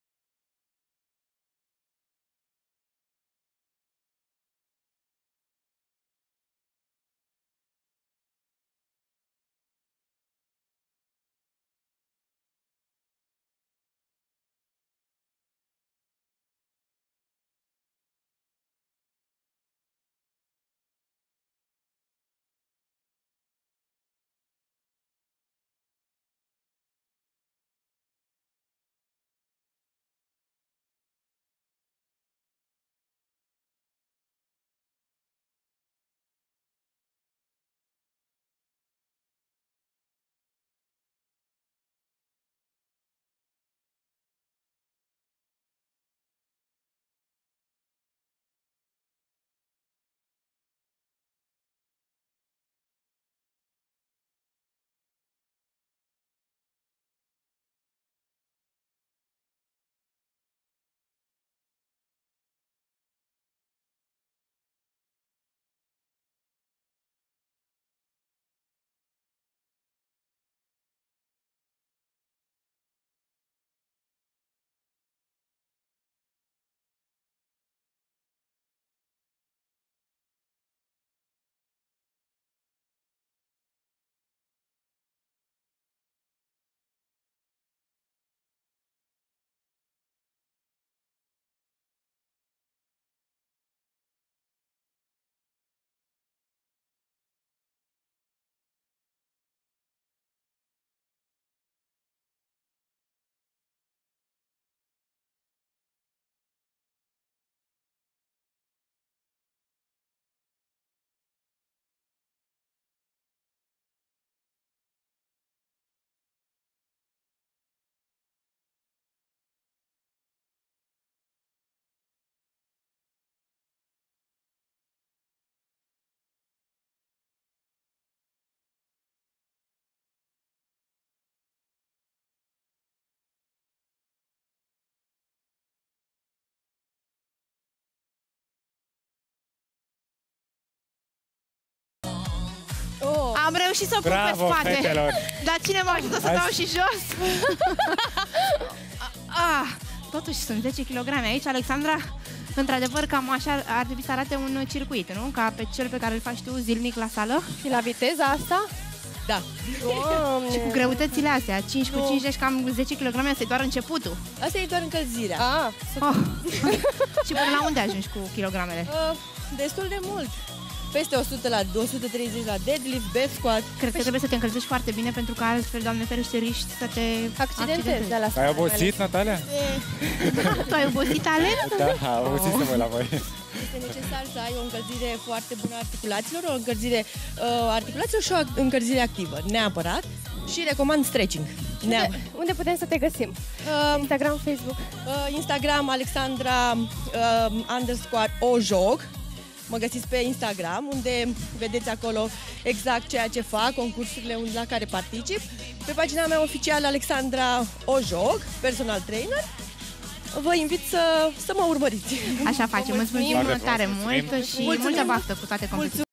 Cum ne antrenăm corect cu cea mai fit femeie din România!
Alexandra Ojog este cea mai fit femeie din România! A născut un băieţel, însă nimic nu a oprit-o să devină una dintre cele mai puternice reprezentante ale sexului frumos din România!